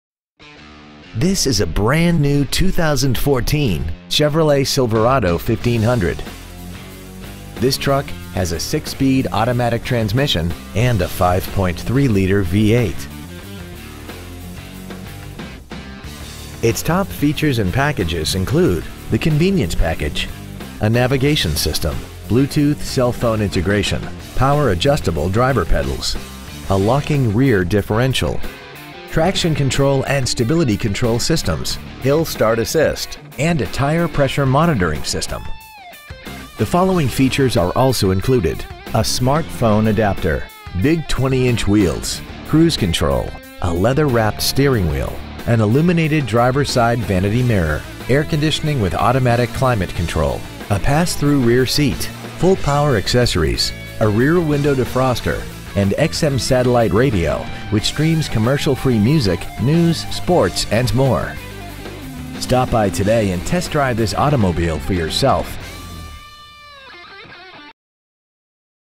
2014 Chevrolet Silverado 1500 Austin TX 78759
We have been honored to serve the Austin TX area , we promise that your experience at our dealership will exceed your expectations ! Year : 2014 Make : Chevrolet Model : Silverado 1500 Engine : Gas/Ethanol V8 5.3L/325 Trans . : Automatic Exterior : White Diamond Tricoat Miles : 6 Interior : Cocoa/Dune Champion Chevrolet Austin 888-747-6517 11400 Research Blvd Austin , TX 78759